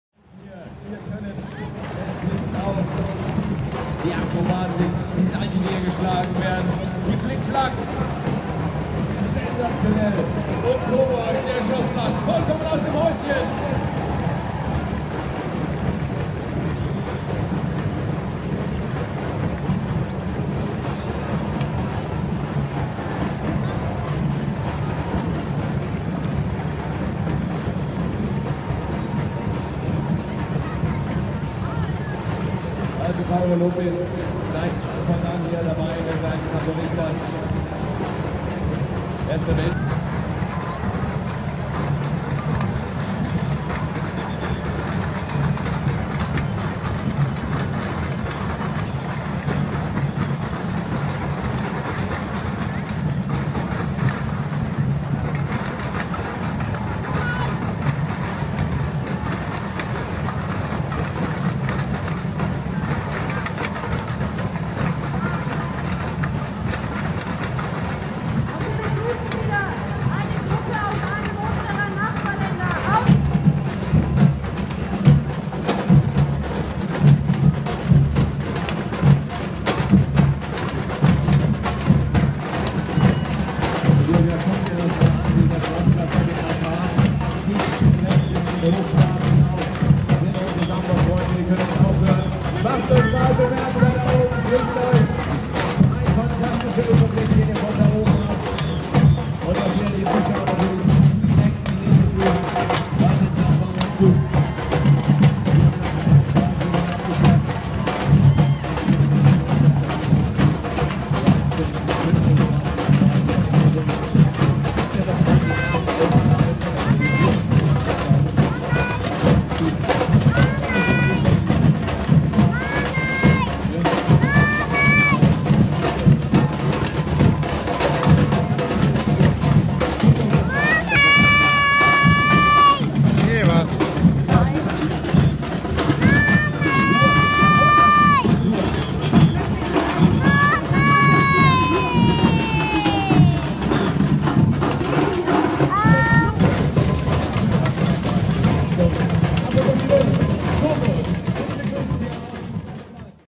Coburg Samba Festival 2016: Bloco Fogo parade
Kent-based band, Bloco Fogo, playing at the Coburg Samba Festival.